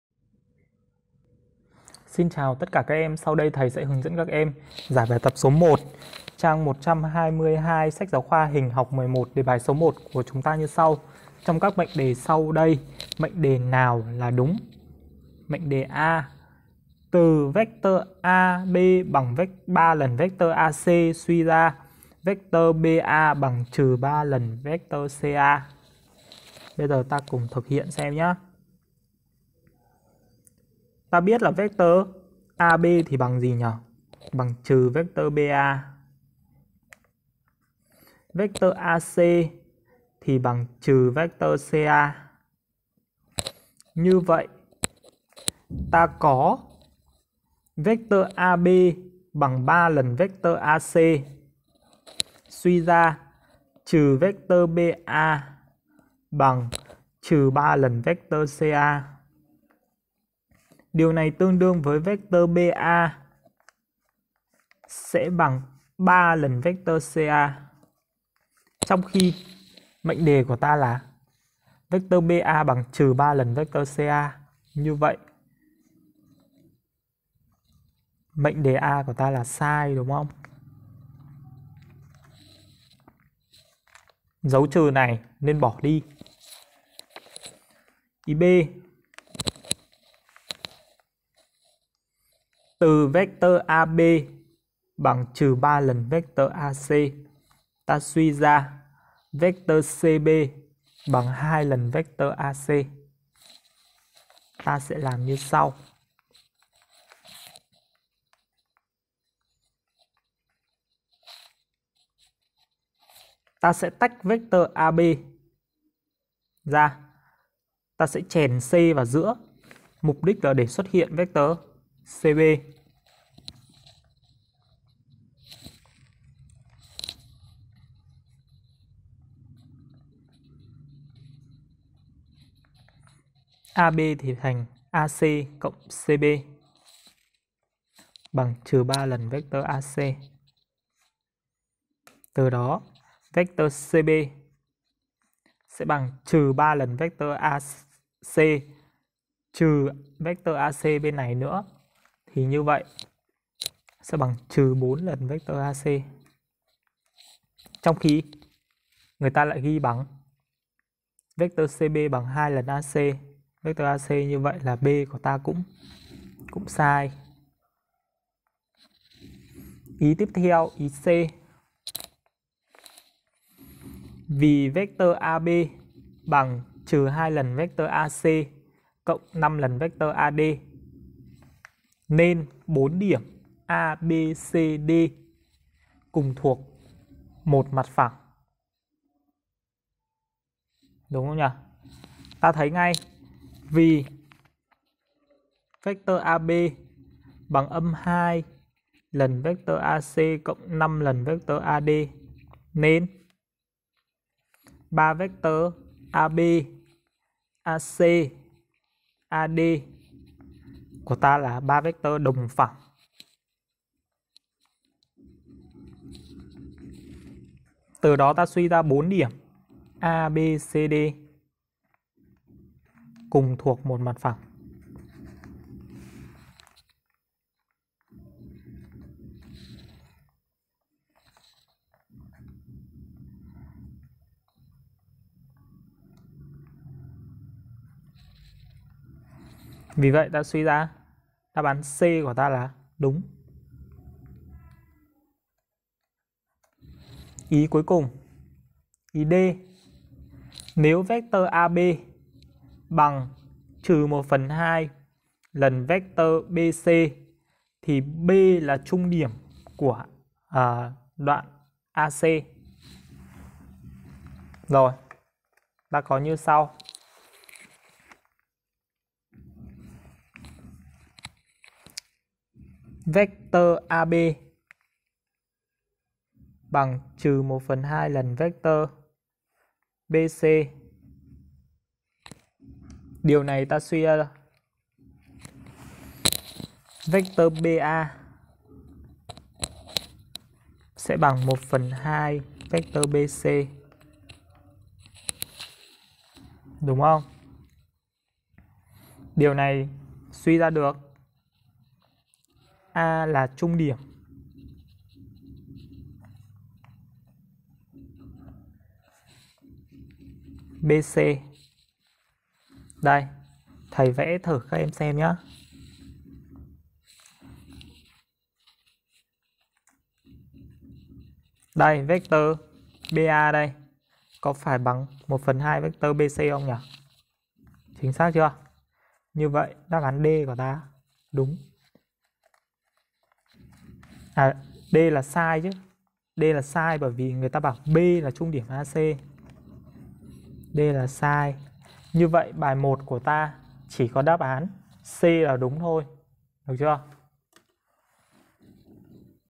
Giải bài 1 trang 122 SGK Hình học 11
Giải bài 1 trang 122  sách giáo khoa Hình học 11 với lời giải chi tiết, ngắn gọn nhất sẽ giúp các em nắm bắt các kiến thức cơ bản và nâng cao một cách nhanh nhất
Trong các mệnh đề sau đây, mệnh đề nào là đúng?
 (A) Từ \(\overrightarrow {AB}  = 3\overrightarrow {AC} \) ta suy ra \(\overrightarrow {BA}  =  - 3\overrightarrow {CA} \)
 (B) Từ \(\overrightarrow {AB}  =  - 3\overrightarrow {AC} \) ta suy ra \(\overrightarrow {CB}  = 2\overrightarrow {AC} \)
 (C) Vì \(\overrightarrow {AB}  =  - 2\overrightarrow {AC}  + 5\overrightarrow {AD} \) nên bốn điểm \(A, B, C\) và \(D\) cùng thuộc một mặt phẳng
 (D) Nếu \(\overrightarrow {AB}  =  - {1 \over 2}\overrightarrow {BC} \) thì \(B\) là trung điểm của đoạn \(AC\) .